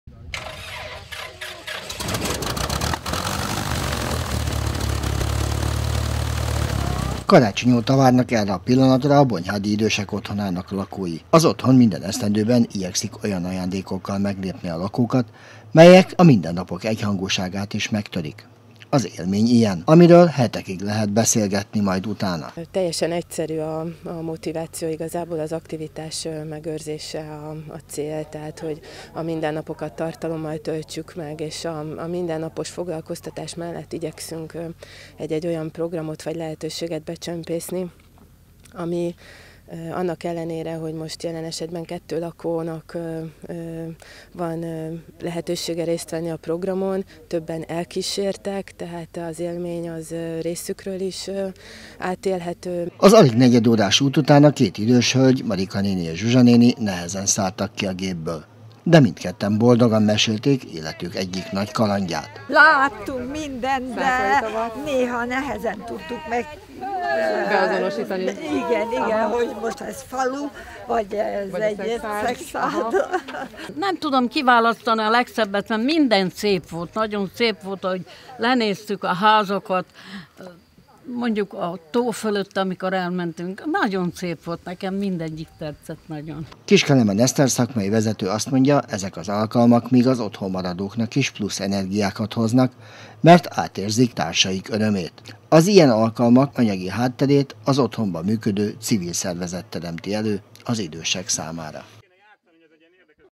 Repülést kaptak ajándékba a bonyhádi Idősek otthonának lakói. 20190508
Repülést kaptak ajándékba a bonyhádi Idősek otthonának lakói. Igaz csak ketten szállhattak a felhők közé de igy is nagy volt az izgalom. azt mondták életük nagy élményét élhették meg.